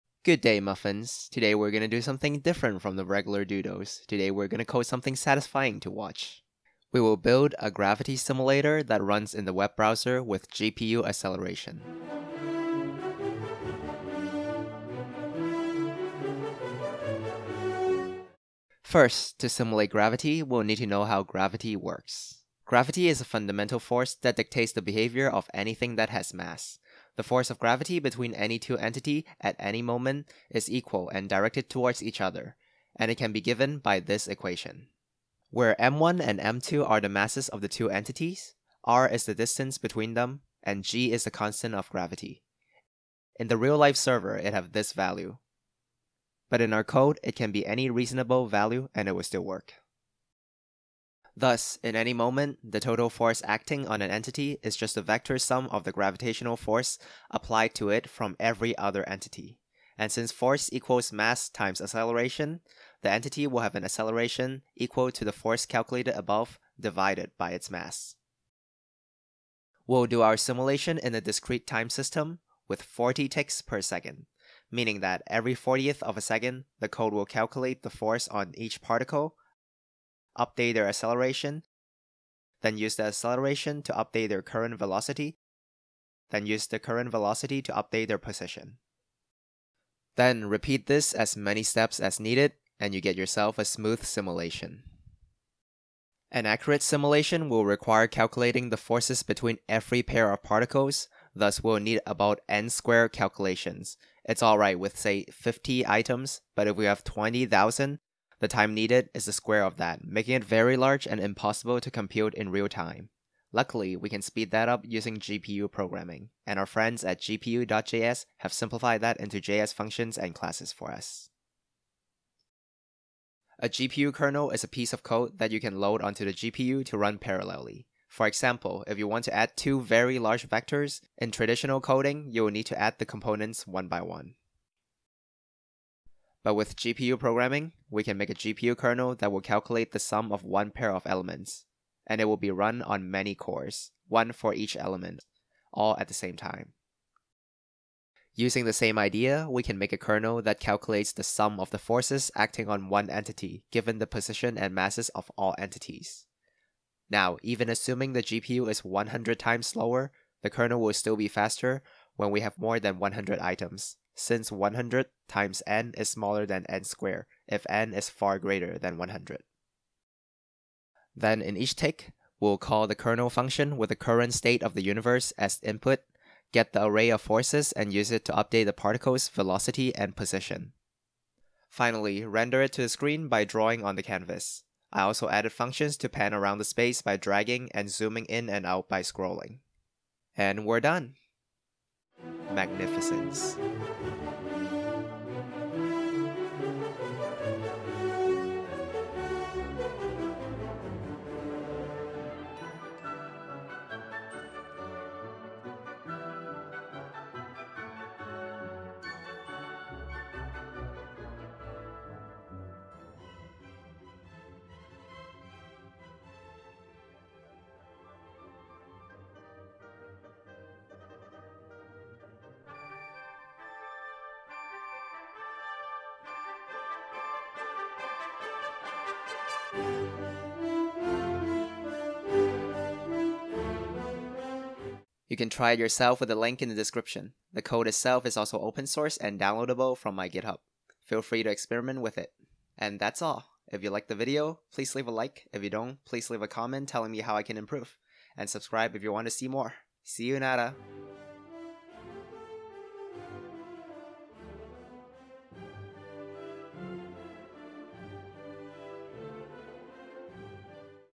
Let's code a satisfying gravity simulator! - Deep Fried Coding ft. Sir Isaac Newton & GPU coding
Kevvol is bored so let's code something satisfying to watch!
How about a fresh mix of gravity with a touch of GPU programming?